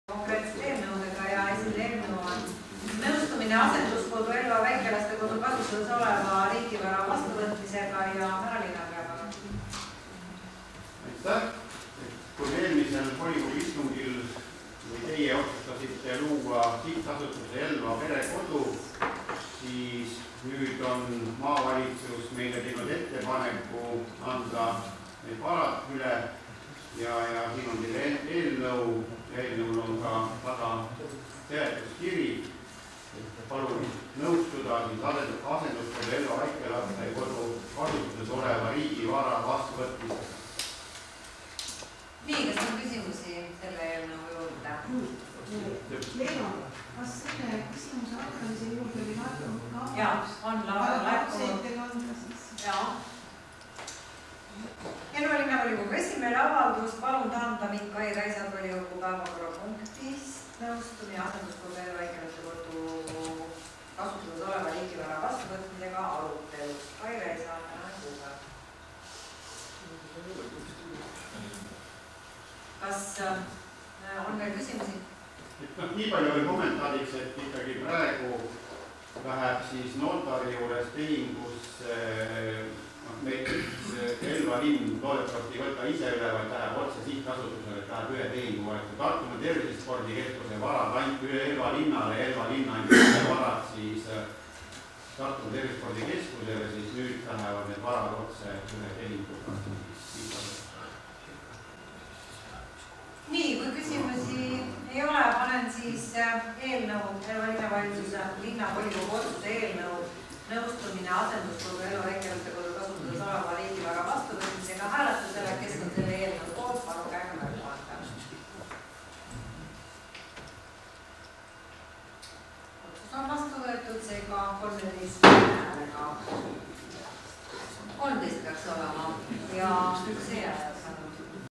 Nõustumine Elva Väikelastekodu kasutuses oleva riigivara vastuvõtmisega
Elva Linnavolikogu istung toimub 24. septembril 2012.a kell 18.00 linnavalitsuse saalis